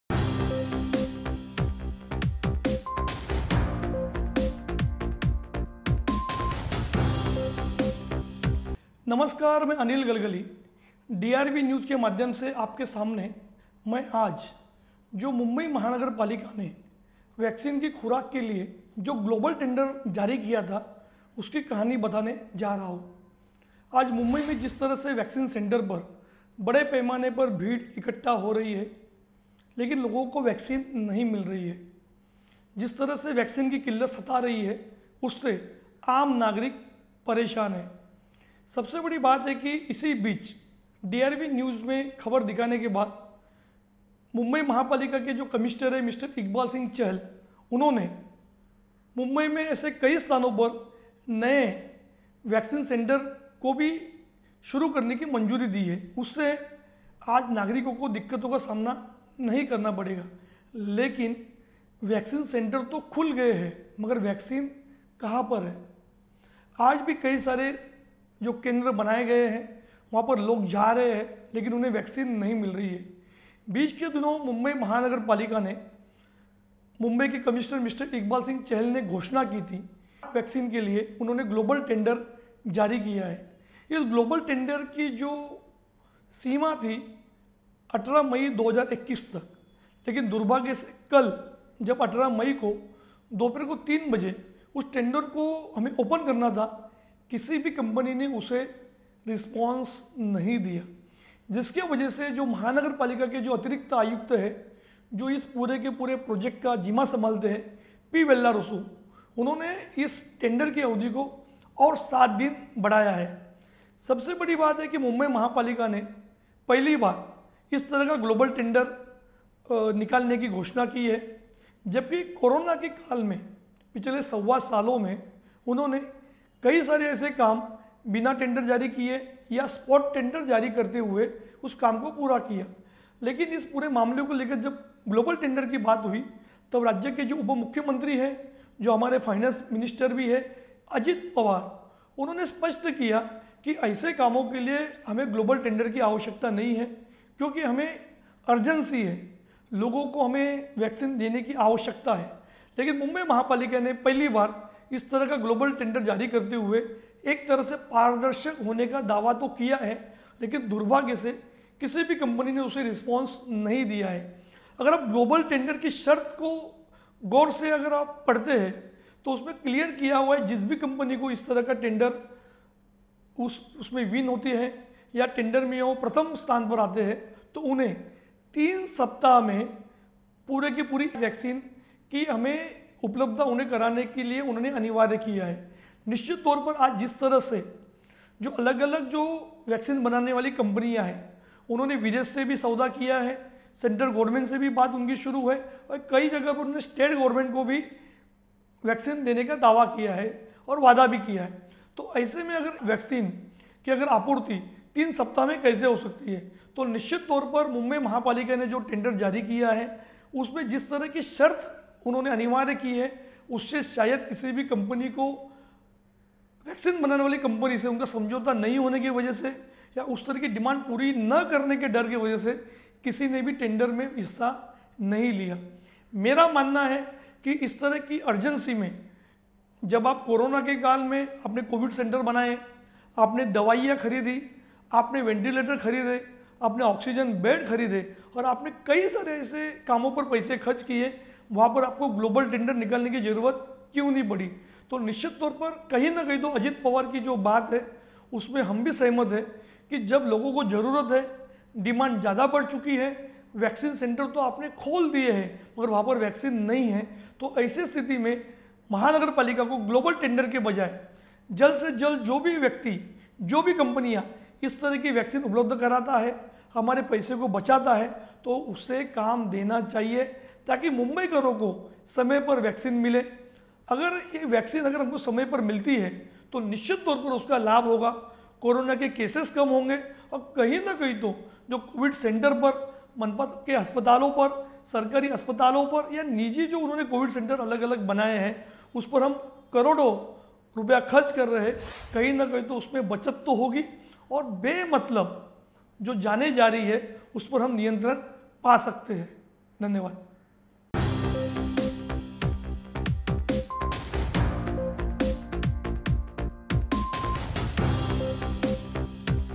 BMC के ग्लोबल टेंडर को आखिर क्यों नही मिला रिस्पॉन्स | DRV NEWS
18 मई तक किसी ने भी नहीं दिखाई रुचि
 25 मई तक बढ़ाई गई नई तारीख
 तीन सप्ताह में देनी हैं 1 करोड़ वैक्सीन
 अजित पवार ने कहा क्या जरूरत है ग्लोबल टेंडर की

  Nobody showed interest till May 18
  New date extended to 25 May
  1 crore vaccine to be given in three weeks
  Ajit Pawar said what is the need for global tender